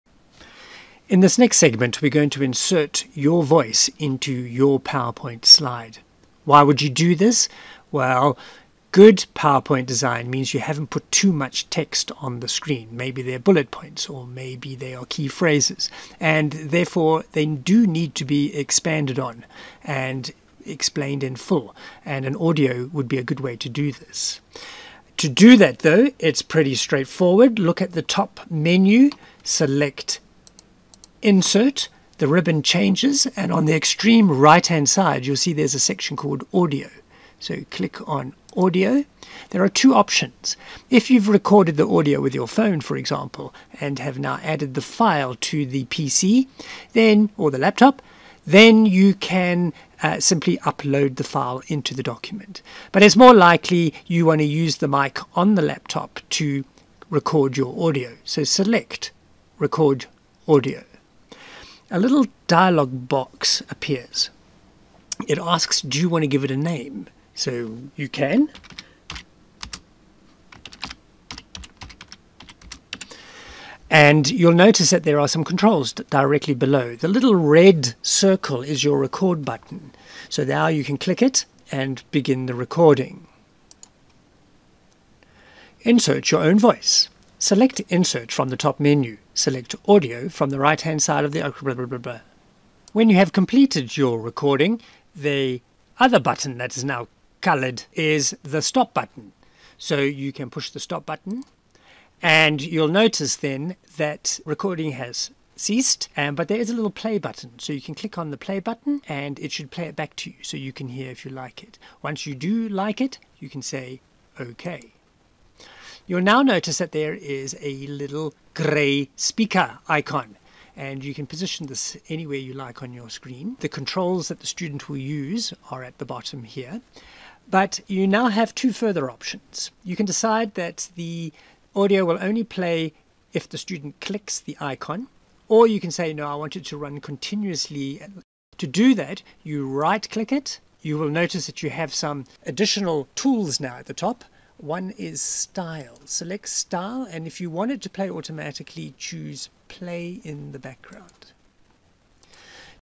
AAU ERT Webinar Series - Insert your own voice into PPT
AAU Emergency Remote Teaching Webinar series - Insert your own voice into your PowerPoint materials